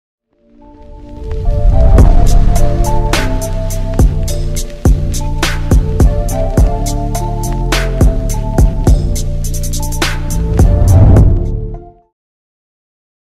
Intro for KaitoHD! 💖🌌 - by MotionUniverse ft. AngelFX (C4D) (My best❤️)
Intro for vivispielt created by MotionUniverse ft. AngelFX (C4D)
In this video I'll show you the spinning text I made for vivispielt...

🎵 Music
➥ Title: NAME

💎 Socialmedia
➥ Skype 📞: MotionUniverse